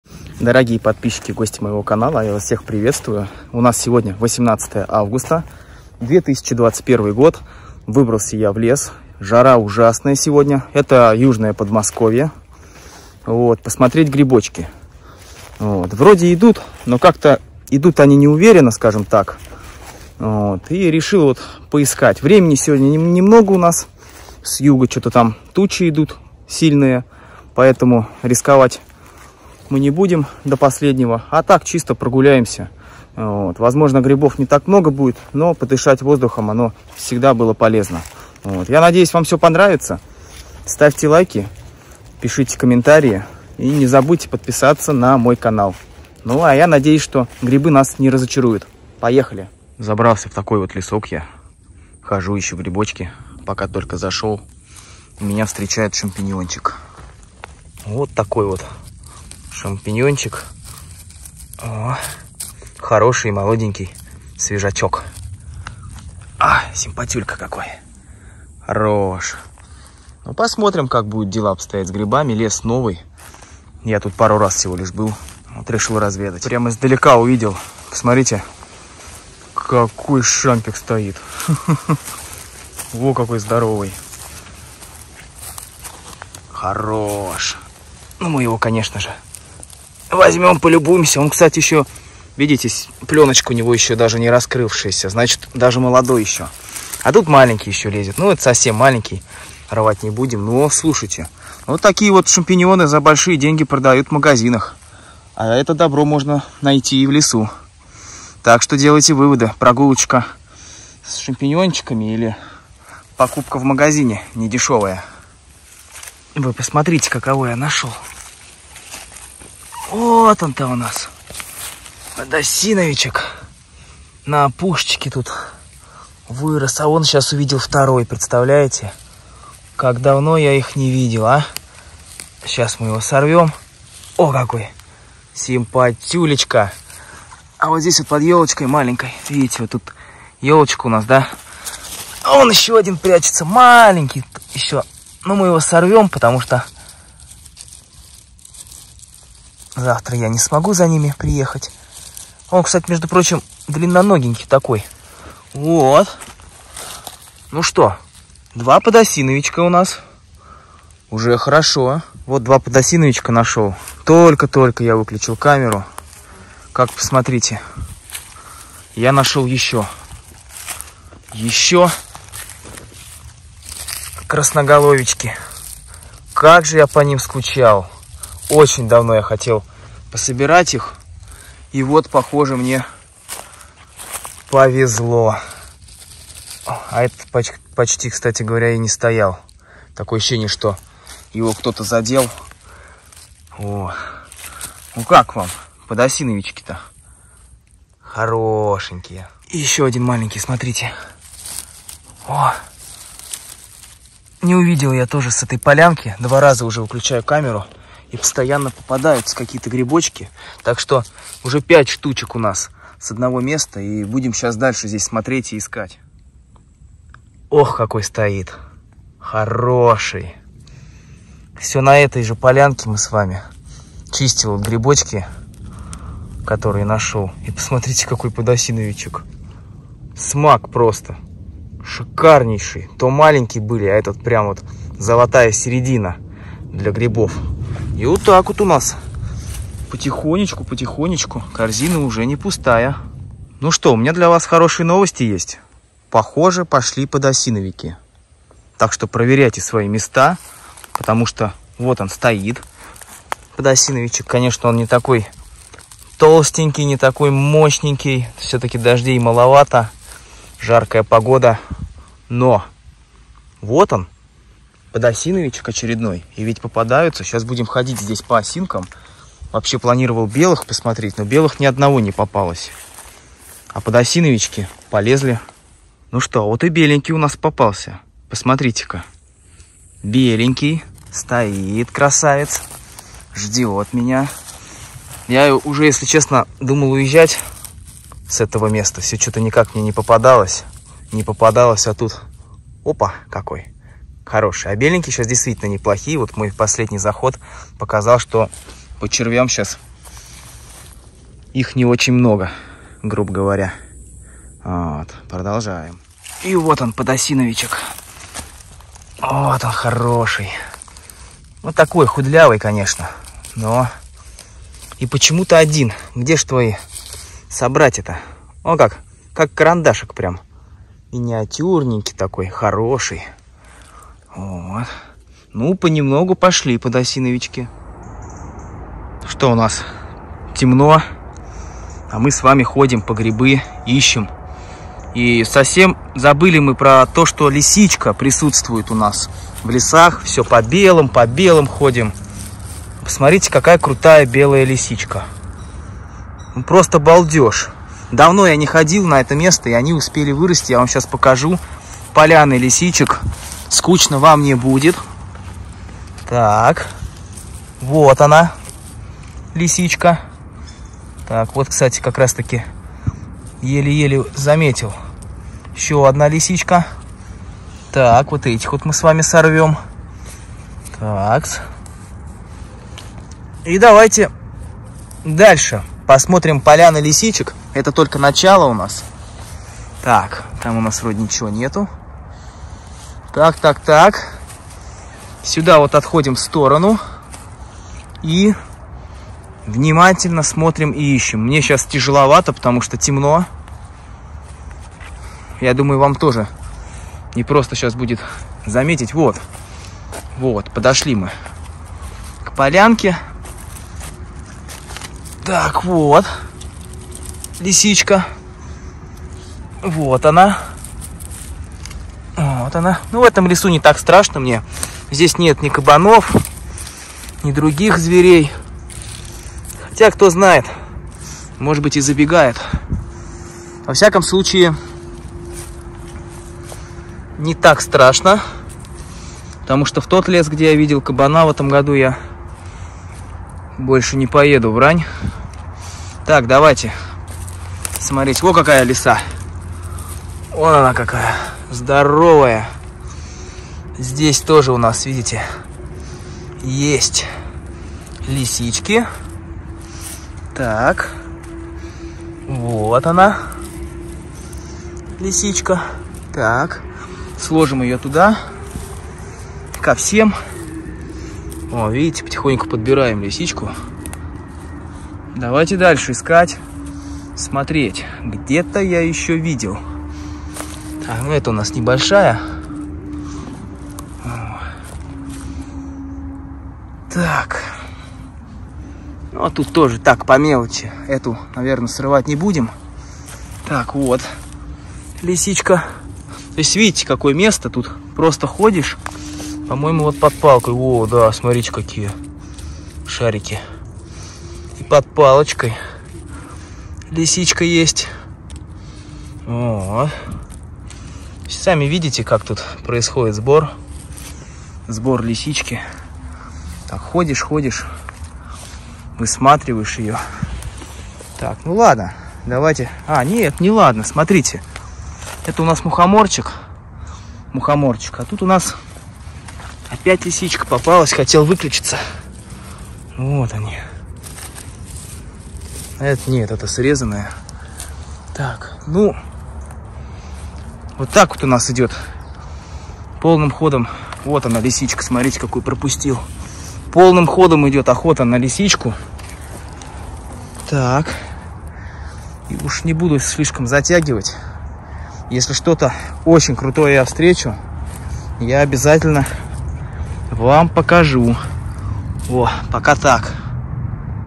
ГРИБНИК ПОШЁЛ В ЛЕС ЗА БЕЛЫМИ ГРИБАМИ А НАШЁЛ ПОДОСИНОВИКИ! ЛИСИЧКИ СОБИРАЕМ ПОЛЯНАМИ!
: Ссылка на прекрасную грибную группу вконтакте